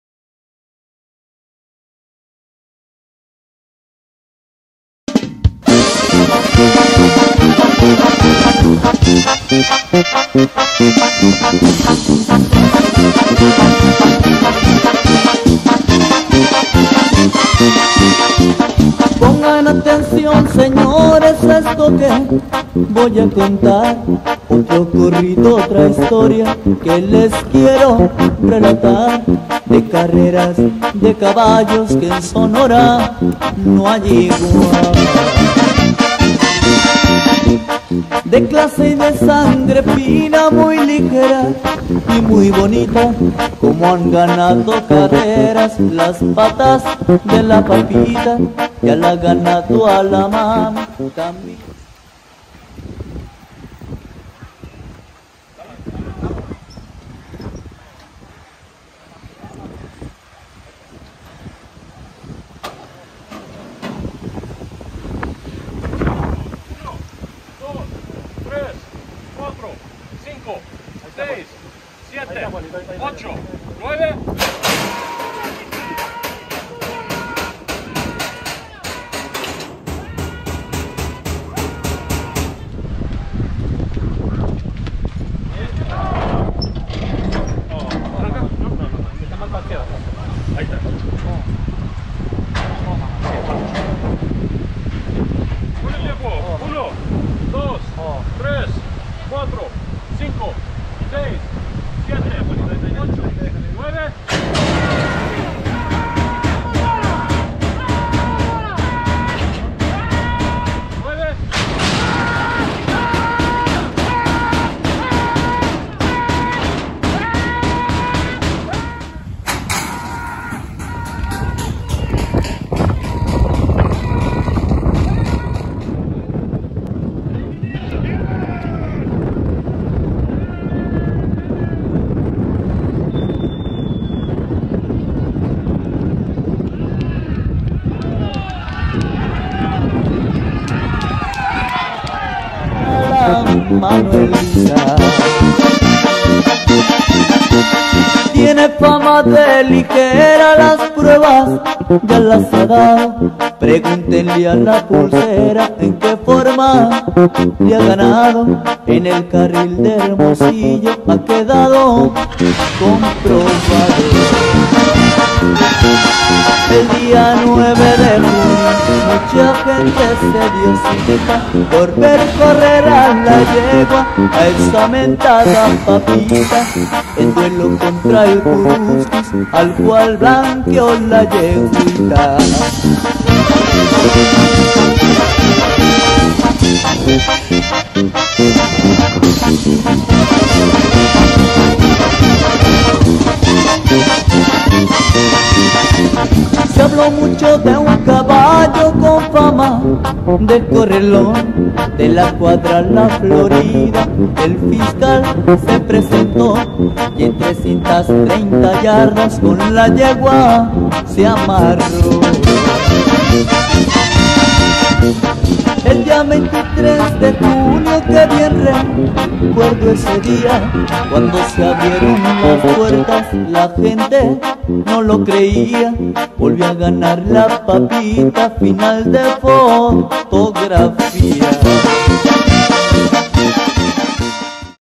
La Basurita Cuadra Poker de Ases vs LA Mora Cuadra San Isidro en Rancho Los Alamos
La Basurita Cuadra Poker de Ases vs LA Mora Cuadra San Isidro en Rancho Los Alamos de Hesperia, California